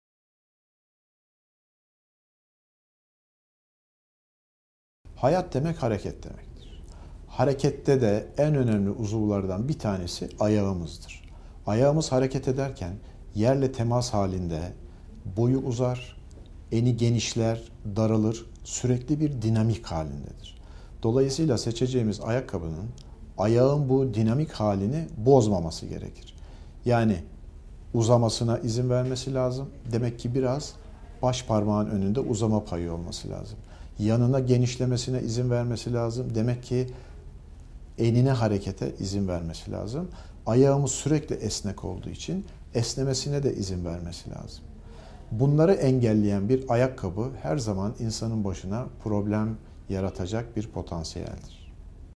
Ayakkabı seçiminde nelere dikkat etmek gerekir?
Acıbadem Kozyatağı Hastanesi, Ortopedi ve Travmatoloji Uzmanı Prof. Dr. Metin Türkmen, Ayak Sağlığı üzerine soruları yanıtlıyor;